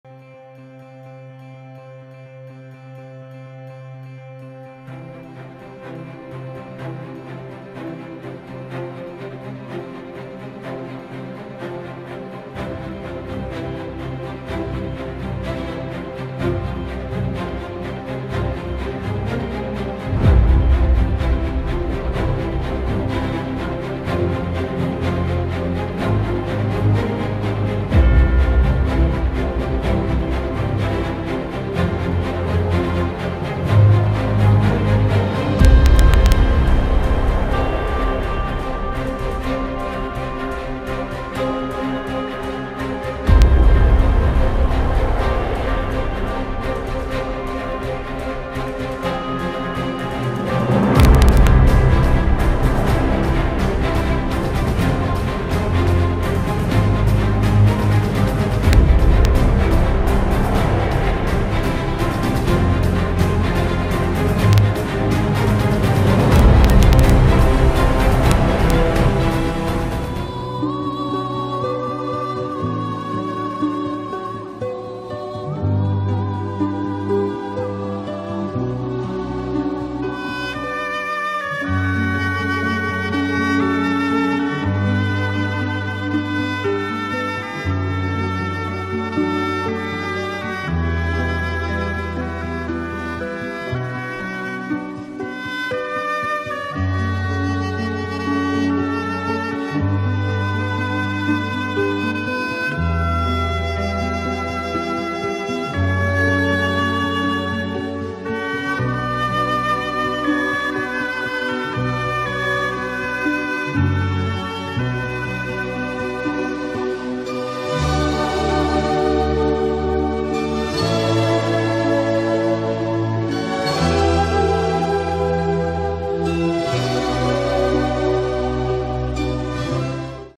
Passeggiata mattutina sulla Rosta a Quinto di Treviso
All'inizio marcio a tempo di musica, ma all'apparire del panorama la musica diventa dolce e rilassante.